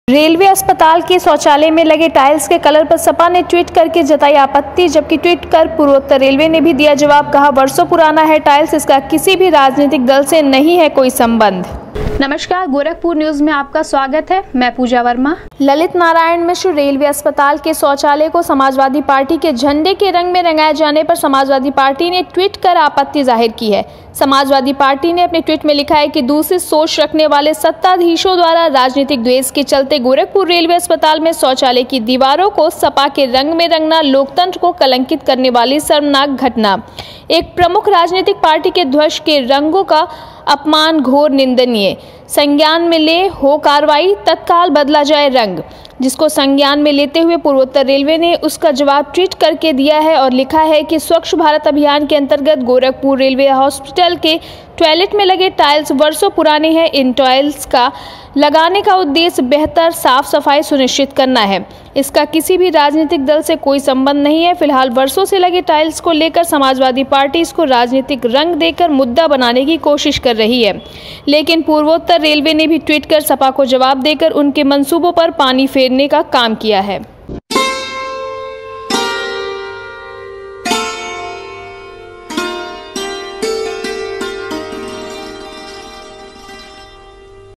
टाइल्स के कलर पर सपा ने ट्वीट के जताई आपत्ति,जबकि ट्वीट कर पूर्वोत्तर रेलवे ने भी दिया जबाब,
रेलवे अस्पताल के शौचालय में लगे टाइल्स के कलर पर सपा ने ट्वीट के जताई आपत्ति,जबकि ट्वीट कर पूर्वोत्तर रेलवे ने भी दिया जबाब, कहा,वर्षो पुराना है टाइल्स , इसका किसी भी राजनीतिक दल से नही है कोई संबंध।।।